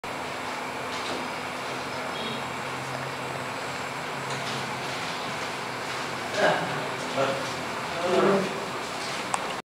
Written Cambodian language
Deaf students practice writing Cambodian language. Wow